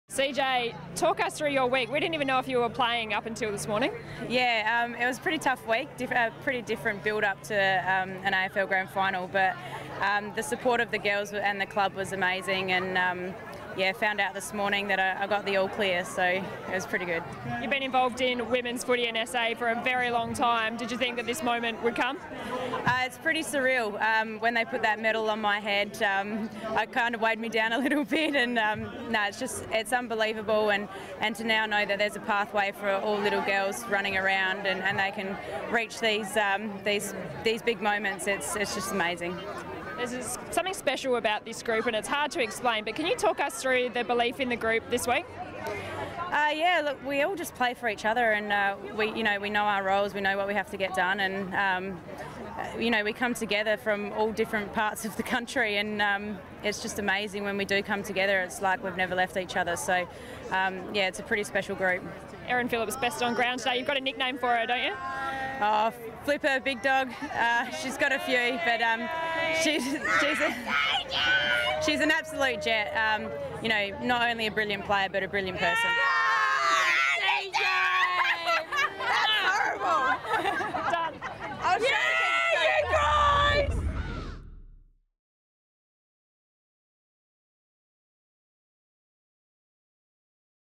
Courtney Cramey Post-Match: GF
Hear from Courtney Cramey after an amazing AFLW Grand Final win.